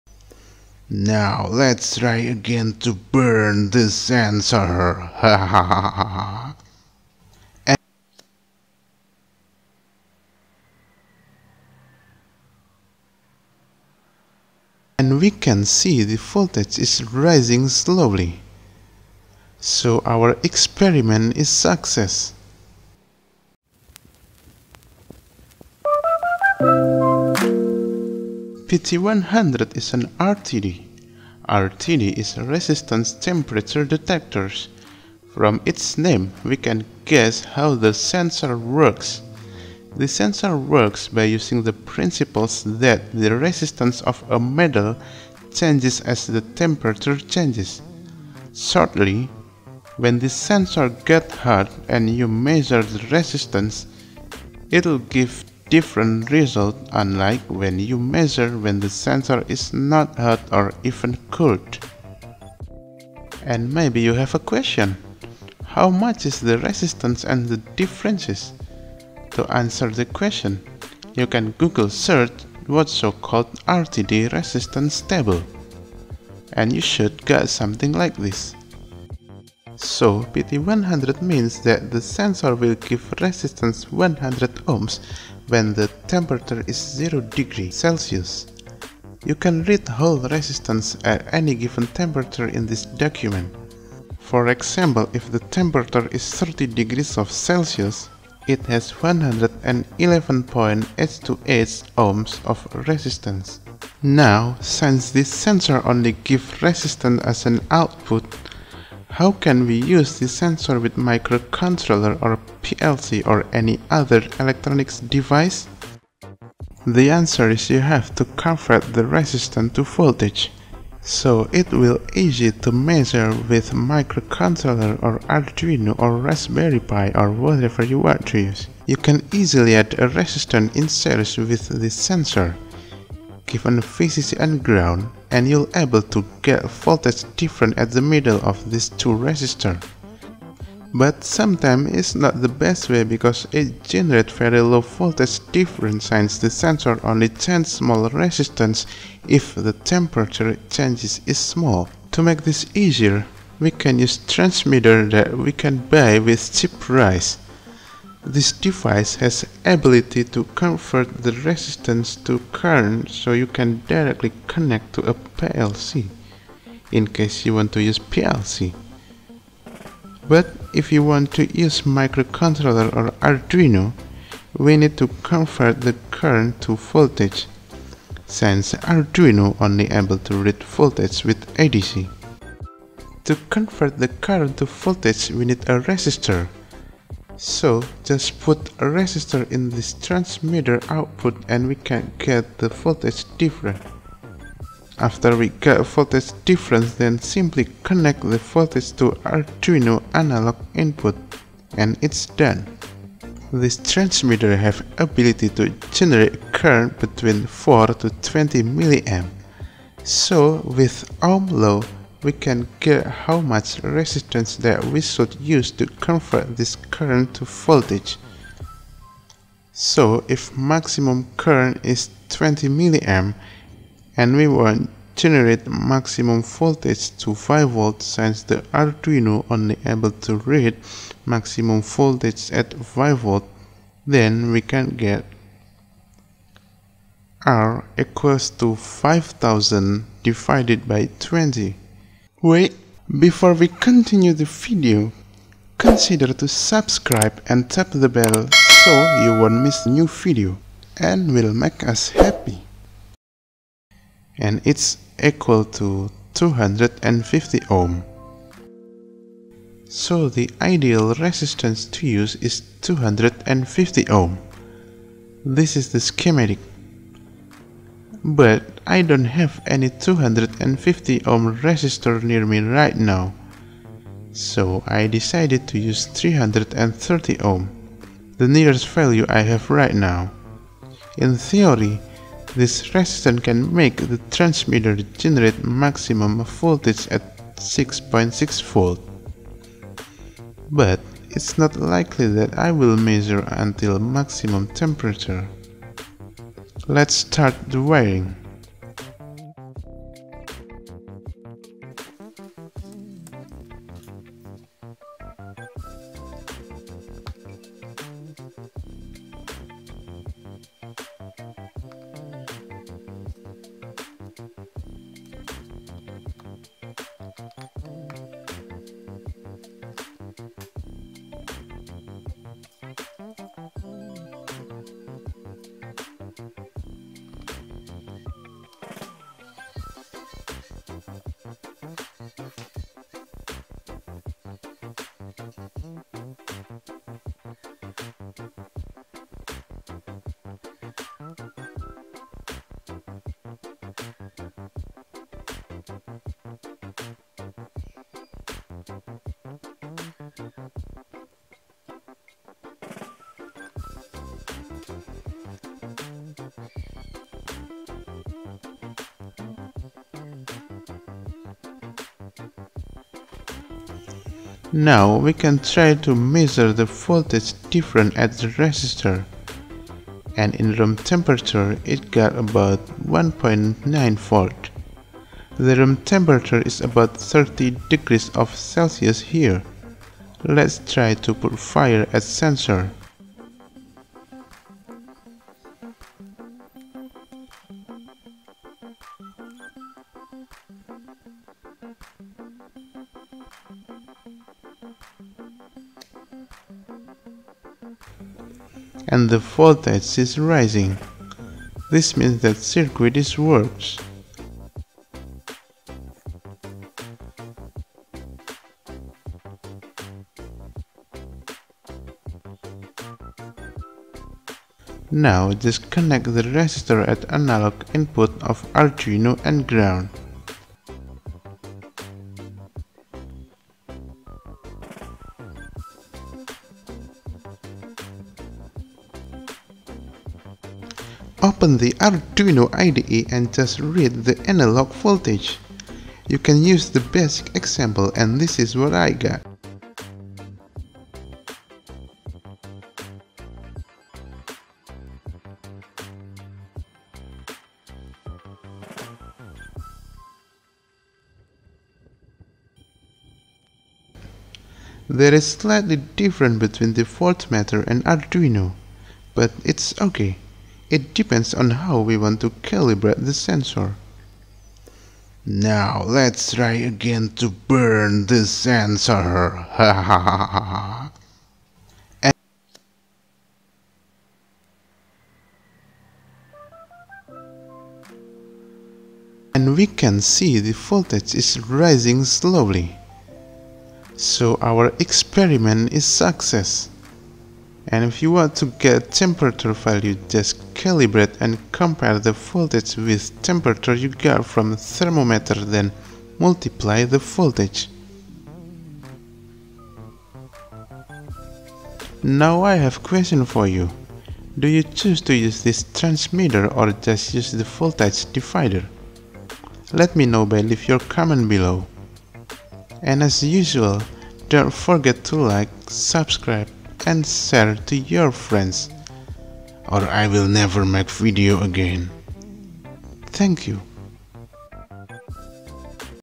How to use RTD sensor ( PT100 with arduino tutorial )
In this video I show you how I use PT100 RTD sensor with an arduino.
RTD is resistance temperature detectors that will output different resistance at different temperature.

The resistance needs to converted to voltage in order to be able to read with an arduino or microcontroller. Watch complete video to know how i do that.